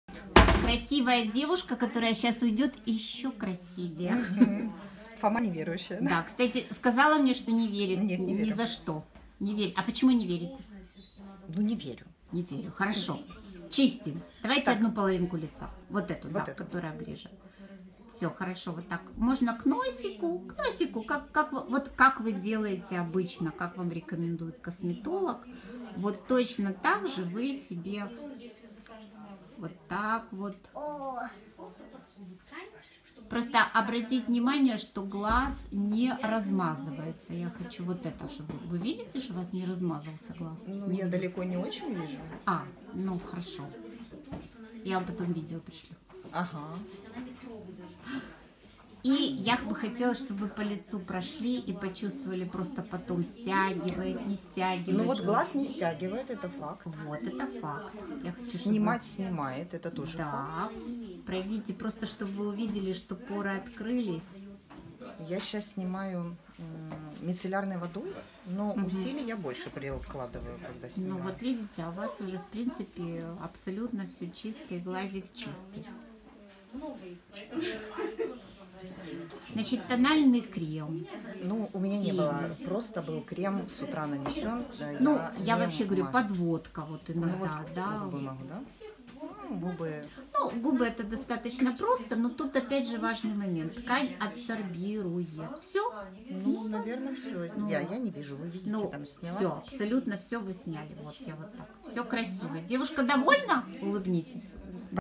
Очищаем лицо от макияжа
Чистик для снятия макияжа абсорбирует водостойкую туш, тональный крем , тени и все это только водичкой и сохраняя красоту вашей кожи